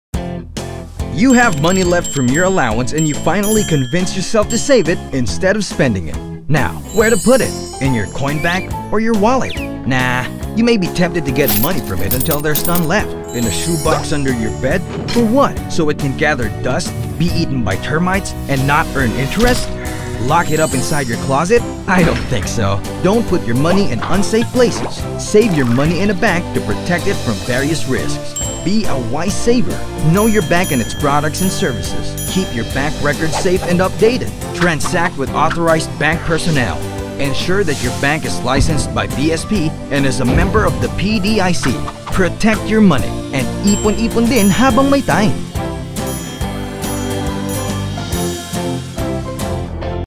Be a Wise Saver
1 of 3: Sharing the iponipondin video campaign produced by BSP in cooperation with the Komisyon ng Seguro, Securities and Exchange Commission and Philippine Deposit Insurance Corporation.